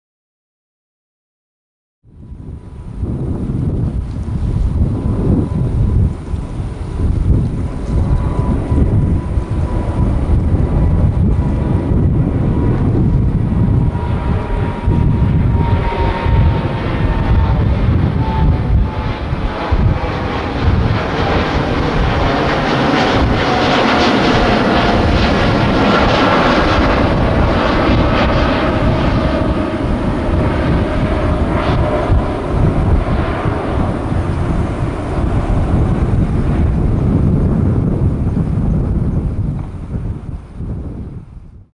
Japan Airlines Boeing 787-9 (JA873J) takeoff from HND/RJTT (Tokyo - Haneda) RWY 34R
Japan Airlines Boeing 787-9 (JA873J) takeoff from HND/RJTT (Tokyo - Haneda Int'l Airport in Japan) on runway 34R.
Date: Sep. 14, 2020 17:18 (UTC+9)
Runway: 34R
Location:
Coordinates for Google Map:
Flight: JL0016 to LAX